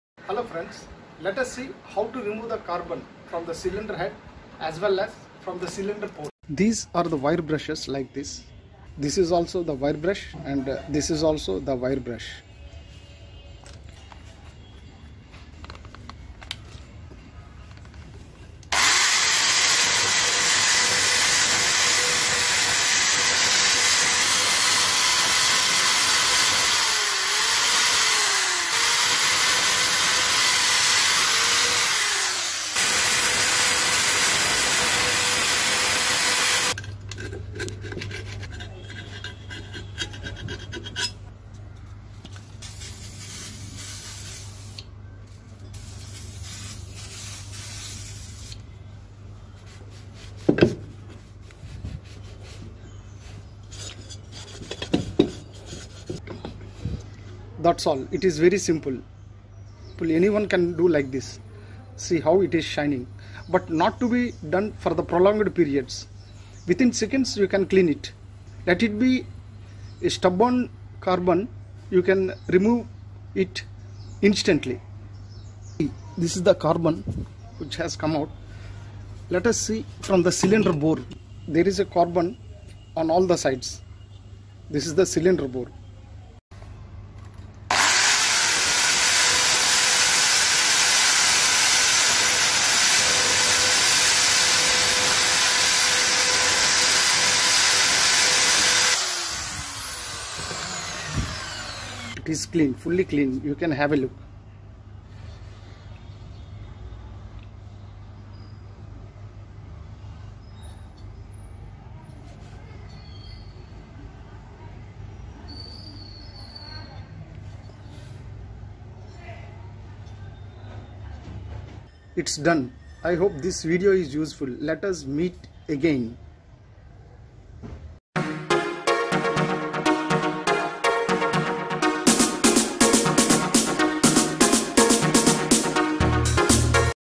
What Are the Best Techniques On How To Clean Carbon Buildup In Engine Like a Pro?
In this informative video, we will explore the best techniques on how to clean carbon buildup in an engine like a pro. Carbon buildup can hinder the performance and efficiency of your engine, leading to decreased power and fuel economy. But fear not, as we will guide you through step-by-step instructions on how to effectively remove carbon deposits and restore your engine's optimal performance. From using specialized cleaning solutions to employing mechanical methods, we will cover a range of techniques that are commonly used by professionals. Whether you are a seasoned mechanic or a DIY enthusiast, this video will equip you with the knowledge and skills to tackle carbon buildup in your engine like a pro. So, join us and discover the secrets to maintaining a clean and efficient engine.